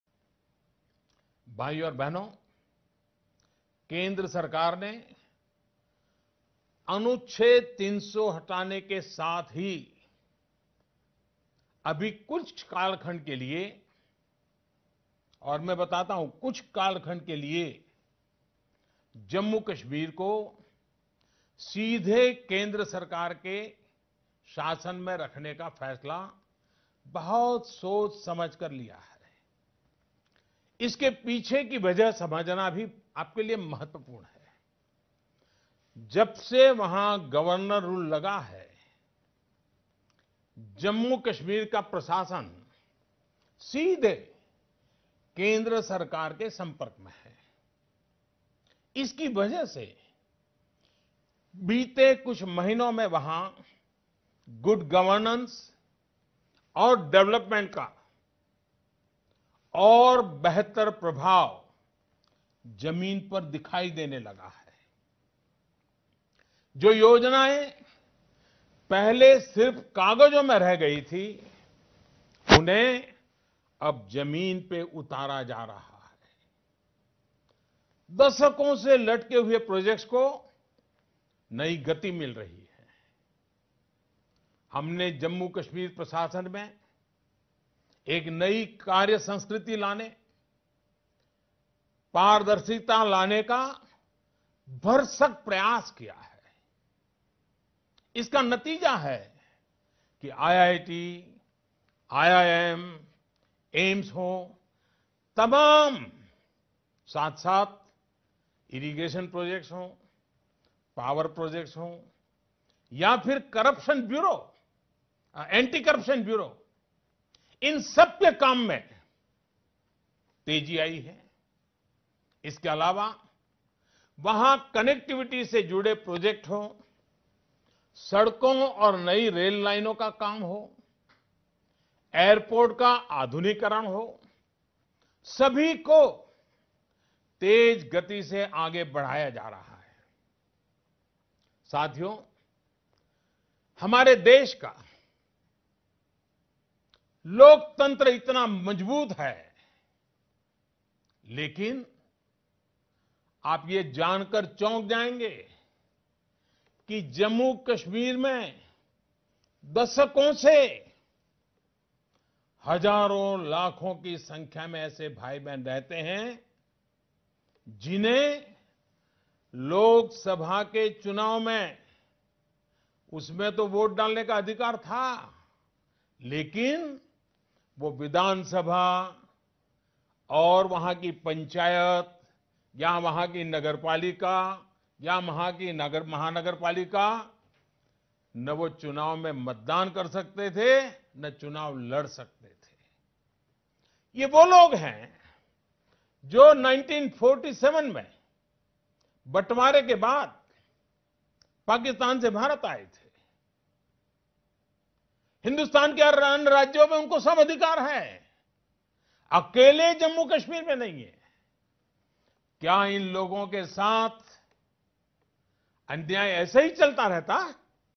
How Modi Govt's good governance is transforming Jammu and Kashmir...Watch video!
Prime Minister Narendra Modi's address to the Nation | 8 August 2019  | Article 370